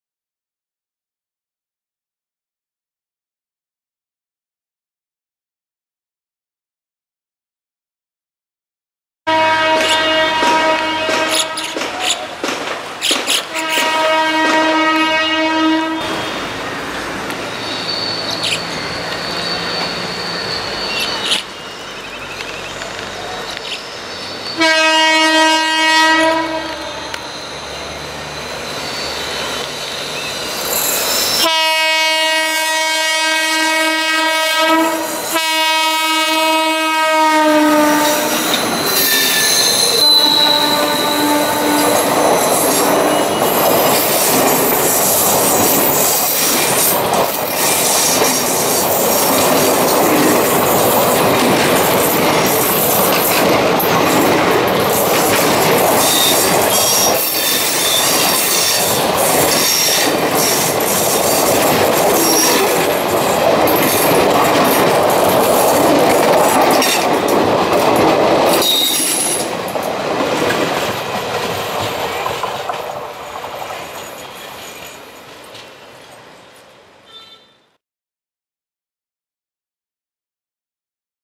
SRC WAP-4 YPR-HWH DURONTO CURVES OUT OF THE GREEN
Red hot SRC WAP-4 the regular link of this train honks as it gracefully curves over towards Banaswadi making a screeching noise from the flanges  hauling the Yesvantpur - Howrah Duronto Express.

This Duronto Express runs 5 days a week in both directions.